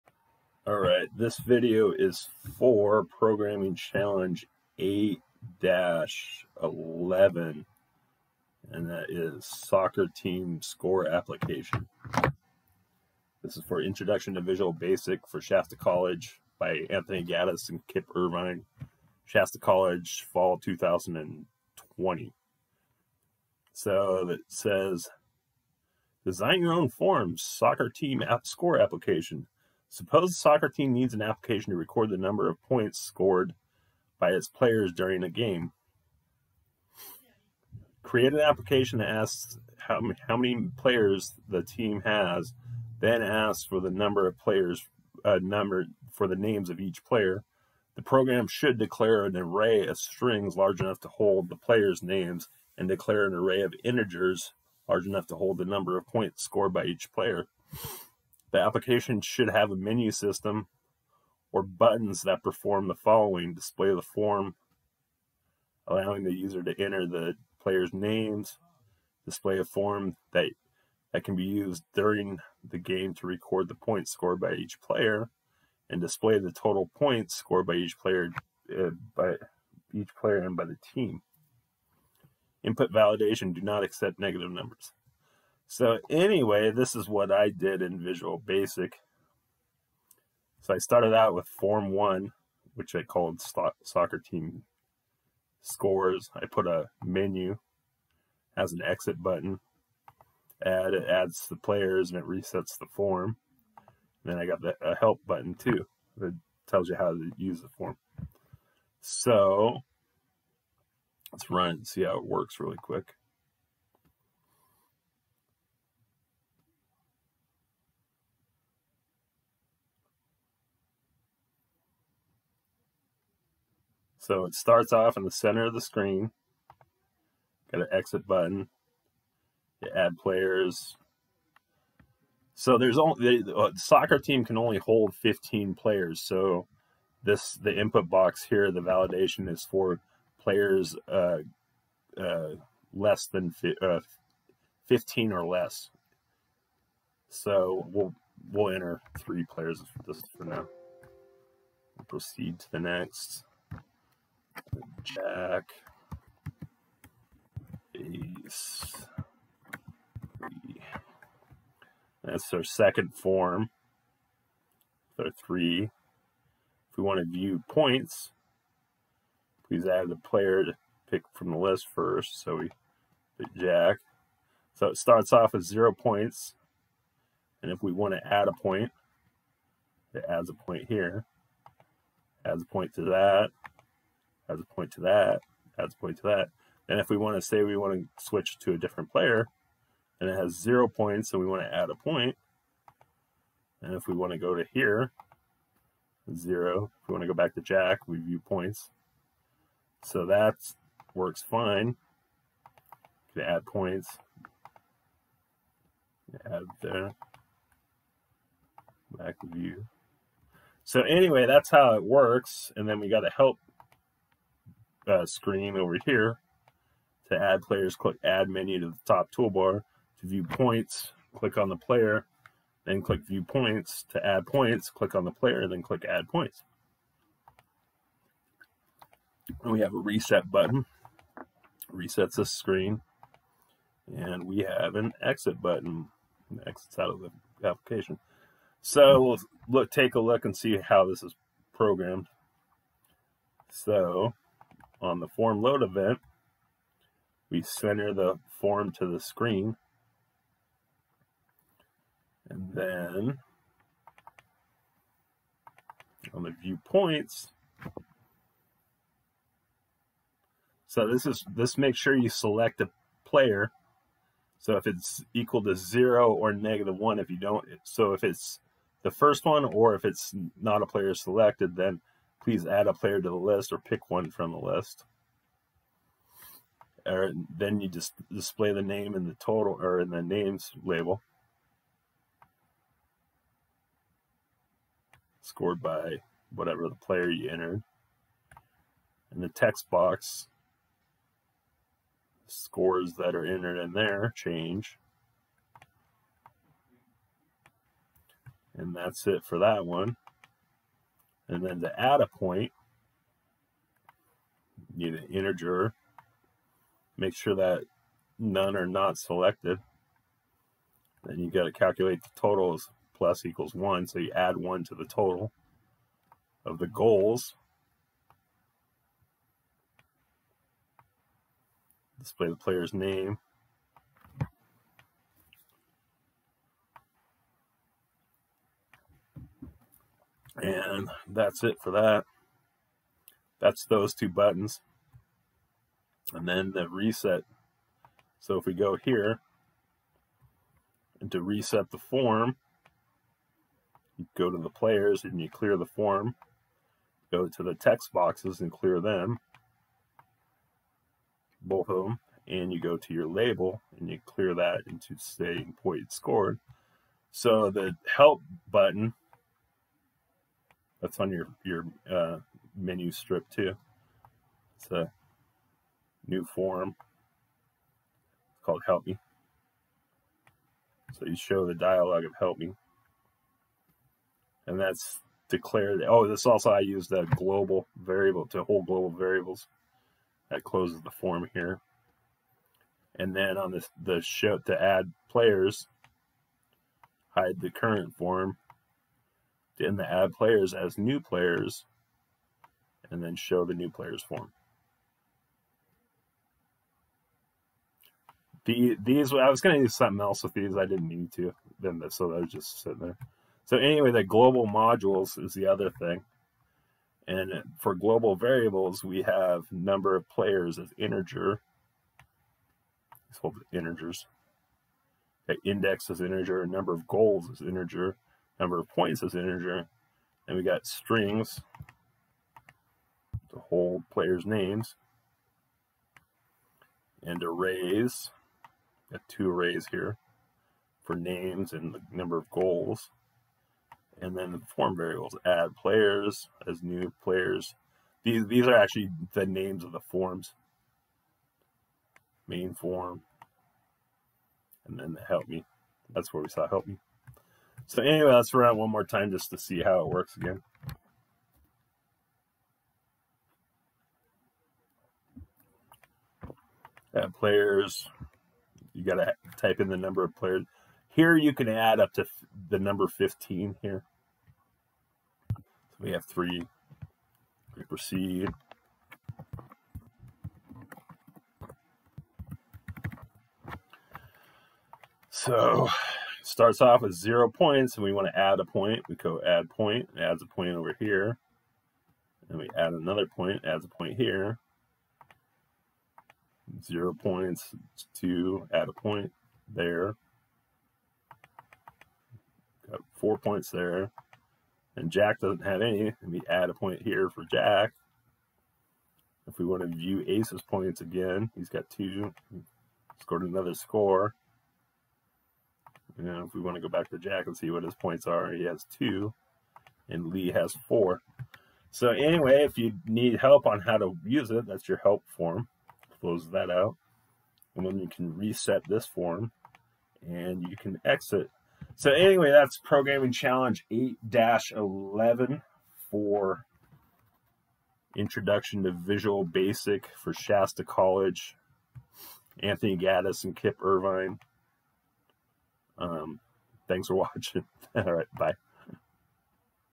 8 11 Soccer Team Score Application Visual Basic 2019
Programming Challenge 8-11 Soccer Team Score Application.  Intro to Visual Basic Shasta College Fall 2020.